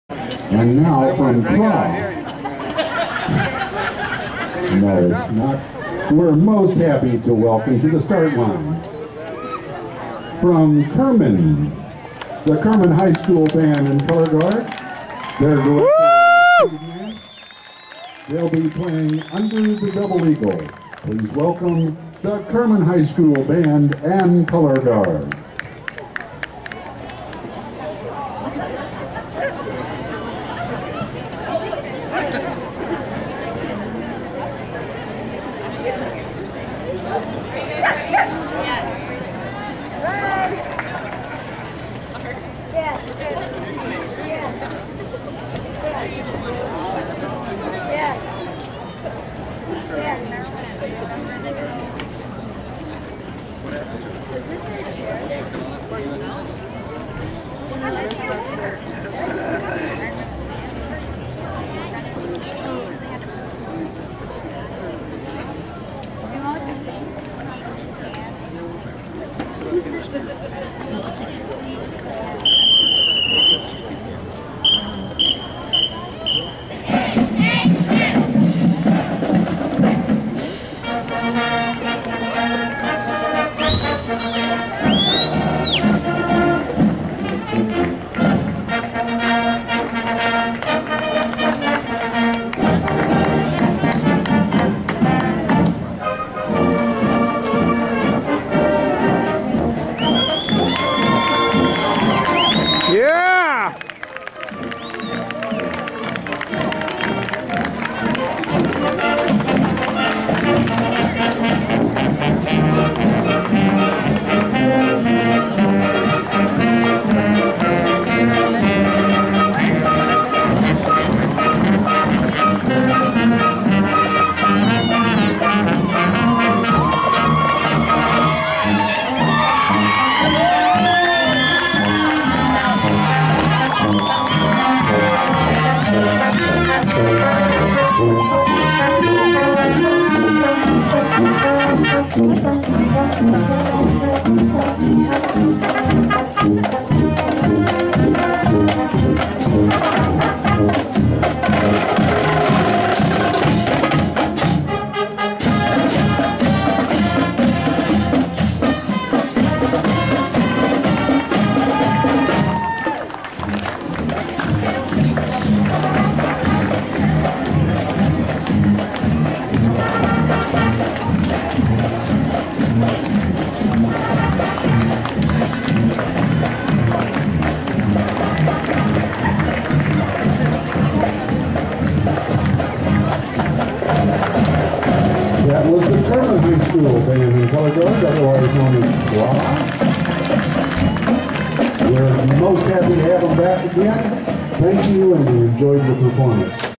KHS Pismo 2010.MPG
Kerman High School Lions Marching Band in the Pismo Beach Vetereans Day Band Review.